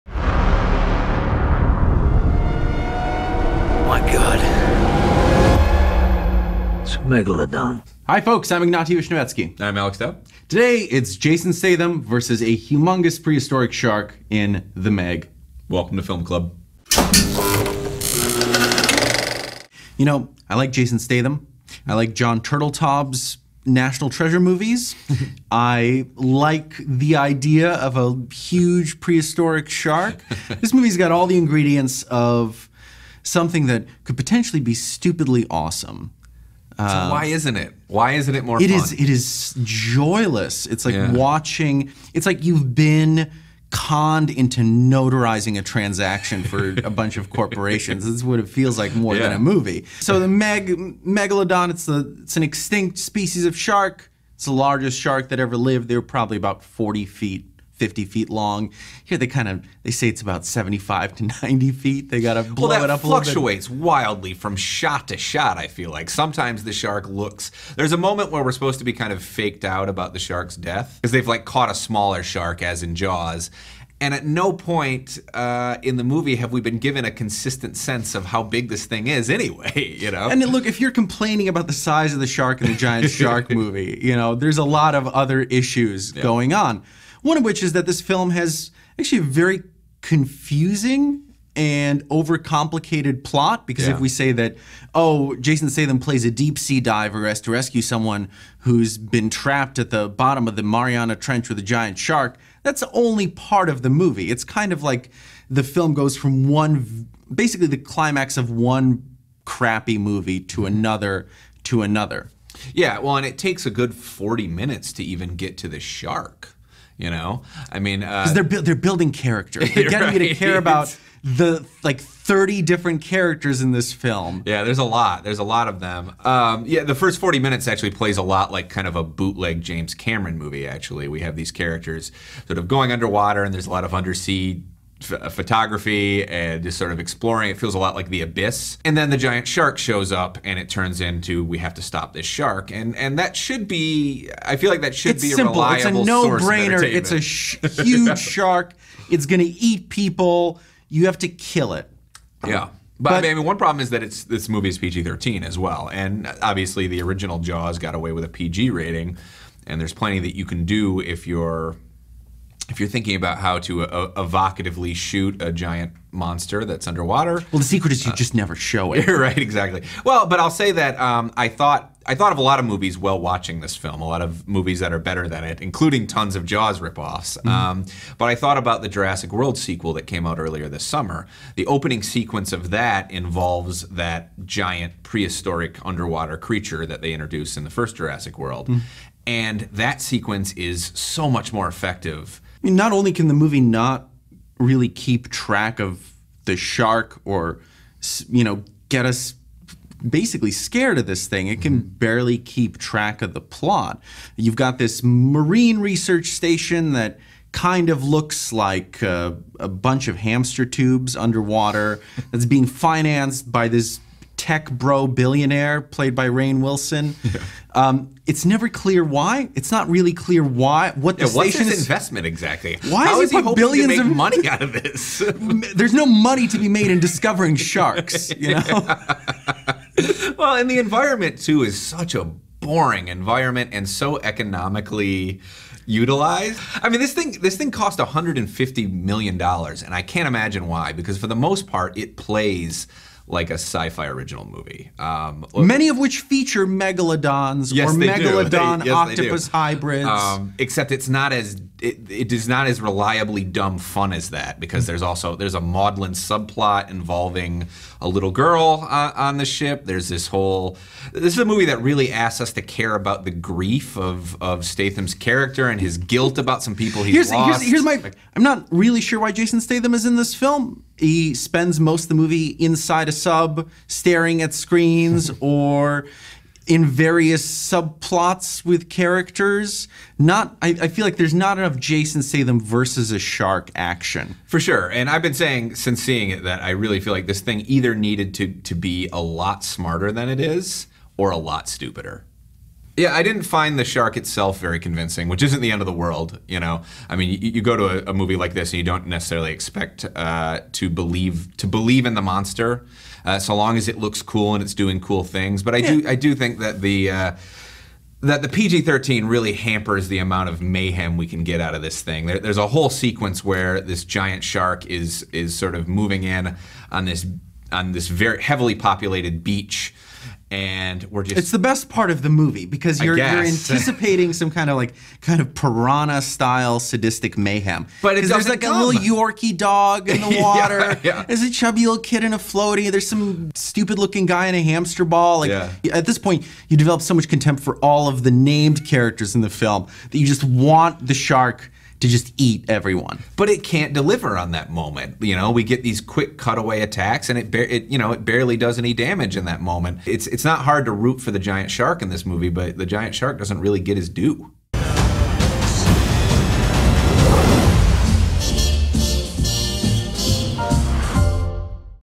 The Meg | Discussion & Review | Film Club
Let’s be reasonable: No one was expecting cinema gold from an August release in which Jason Statham fights a big prehistoric shark. But The Meg still failed to live up to our low expectations. Critics A.A. Dowd and Ignatiy Vishnevetsky sit down to discuss why on the latest Film Club.